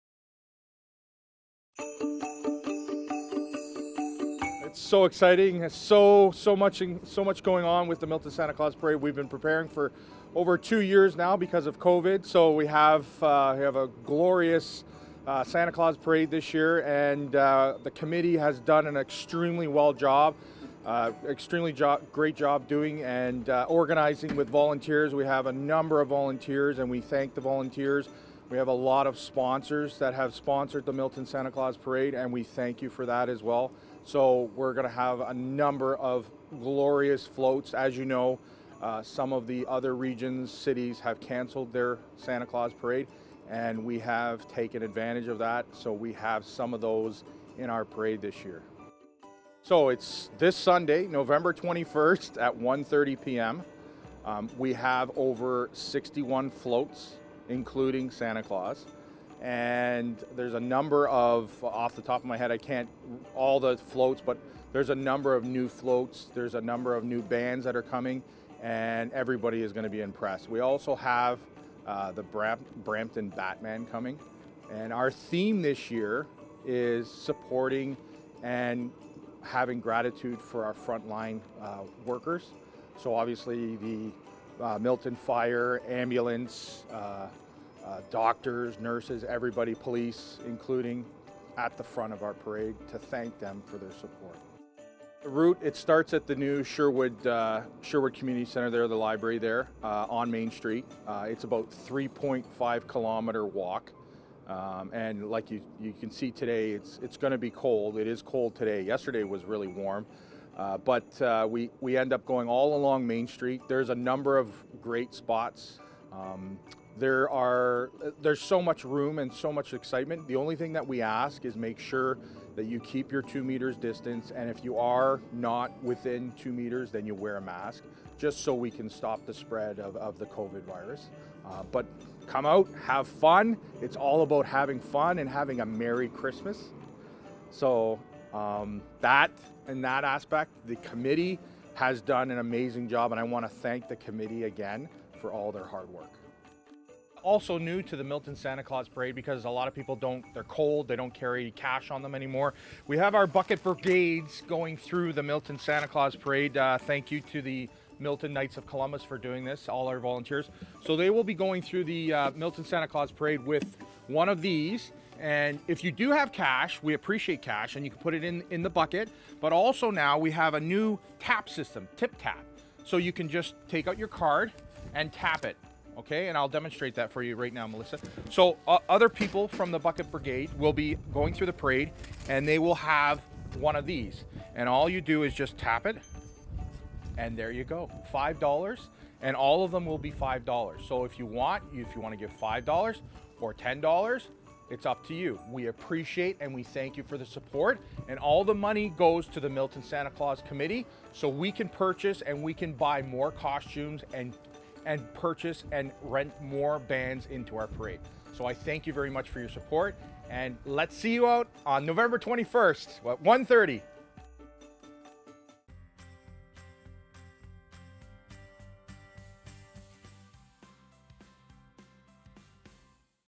Milton Santa Claus Parade a go
The 2021 Milton Santa Claus Parade on Sunday Nov. 21 promises to be bigger and better this year with a new way to donate! It starts at 1:30pm! For details go to miltonsantaclausparade.ca.